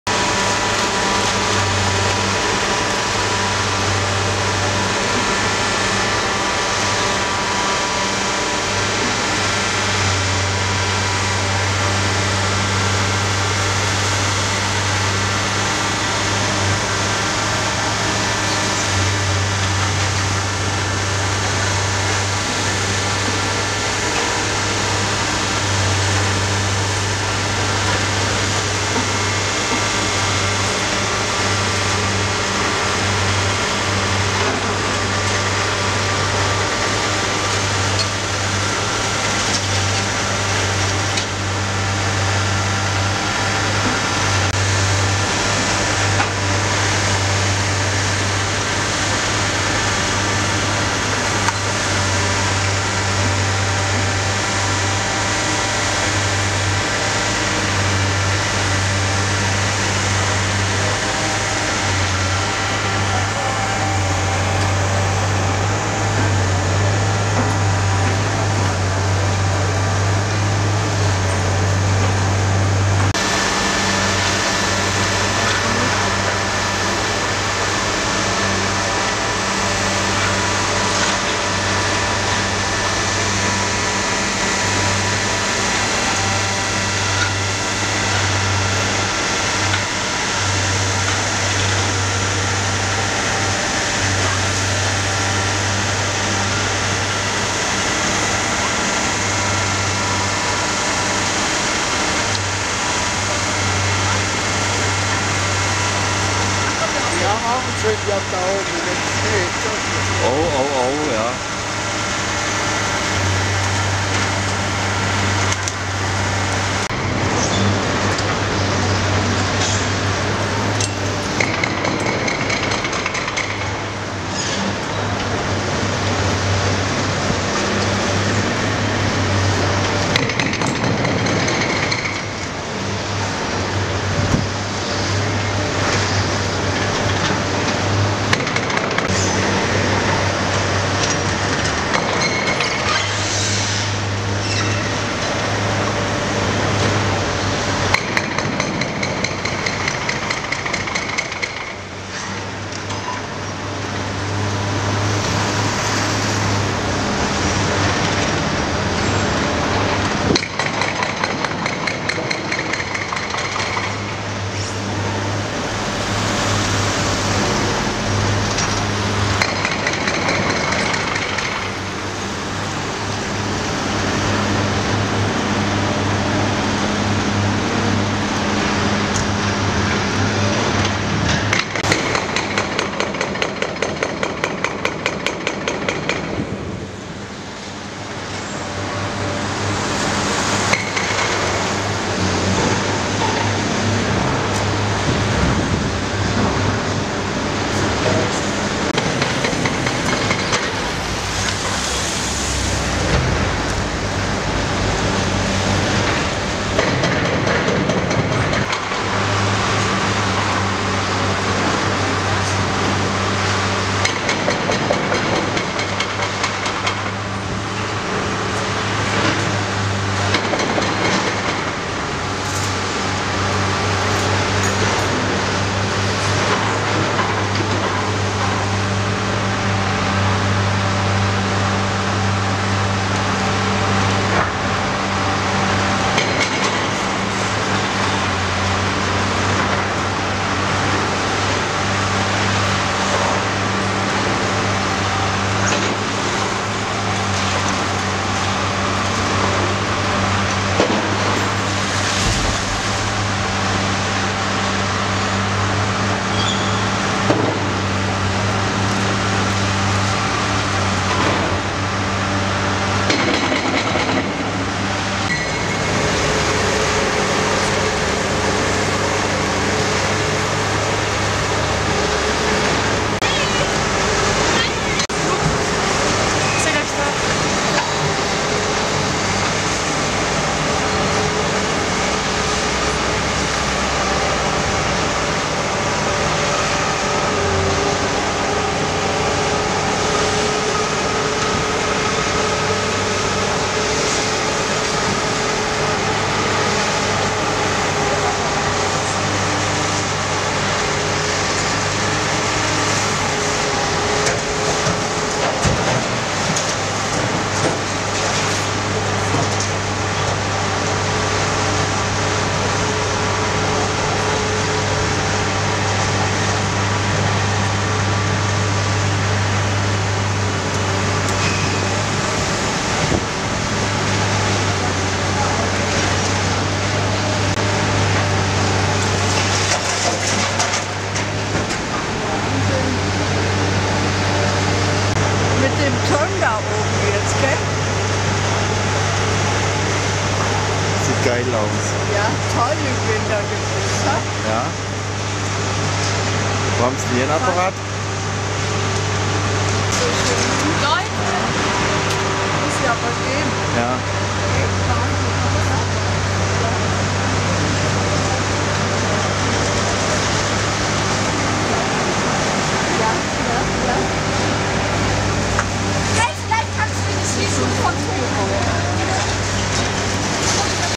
Joseph Pschorr Haus - Die Baustelle am 24.05.2011. - 25.01.2011
Joseph Pschorr Haus. Auch bei Schnee gehen die Abrissarbeiten des alten Karstadt am Dom auf volle Touren weiter.